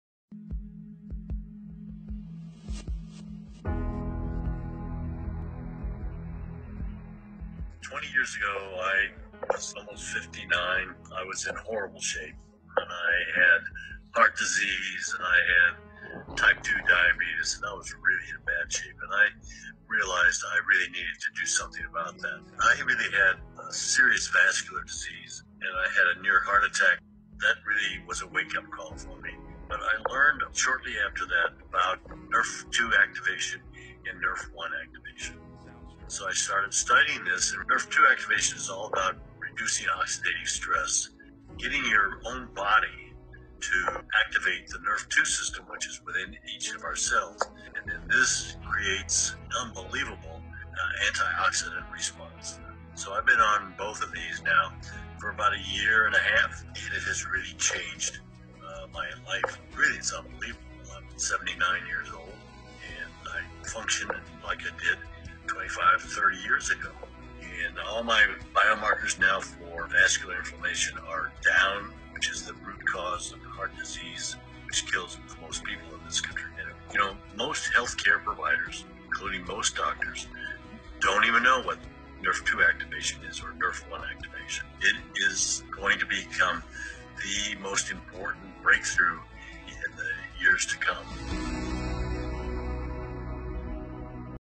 What does Dr Jeffry Life have to say about Protandim Nrf2 Activation
In Dr Life's own words

"As a leading anti-aging, prevention-focused physician, winner of the 2012 Top 25 Fittest Men by Men’s Fitness Magazine, the 1998 Body-for-Life Contest and a NY Times Best Selling Author…I have been asked to endorse hundreds of supplement and healthcare products over the years. I do take the time to review new products, but all had failed my test and endorsement, until 2016, as they did not have the science or research to support their marketing claims.

This changed when I was asked to review Protandim Nrf1 and Protandim Nrf2 activators in 2016. I conducted the necessary due diligence and performed a thorough review of the science behind Nrf1 and Nrf2 activation, and reviewed the 23 peer-reviewed Protandim Nrf2 studies, most of which were independently funded by prestigious organizations such as the American Heart Association, National Institutes on Aging, and Harvard and many other research institutions around the world. In addition, Protandim Nrf2 had been featured on major news programs, such as: ABC, NBC, PBS, The Today Show, Wall Street Journal and more."